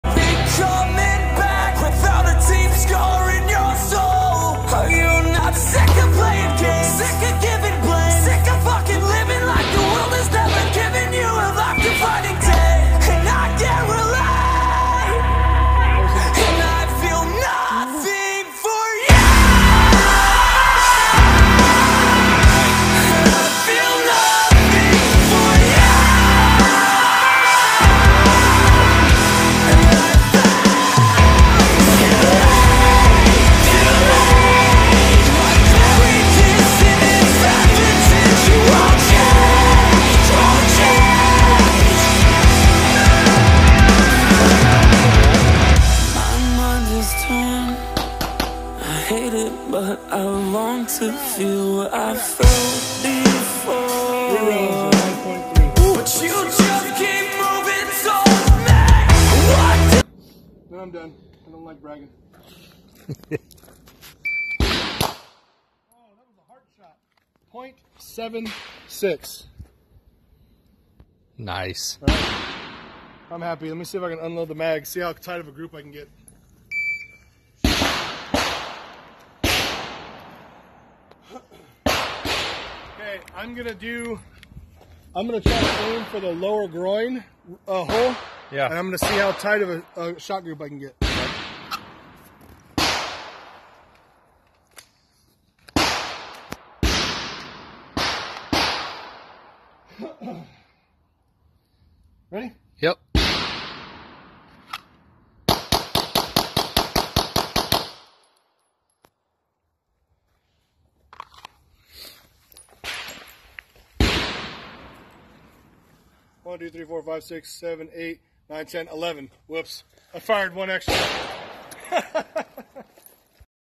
Shot timer work!
Working off a shot timer, trying to get faster.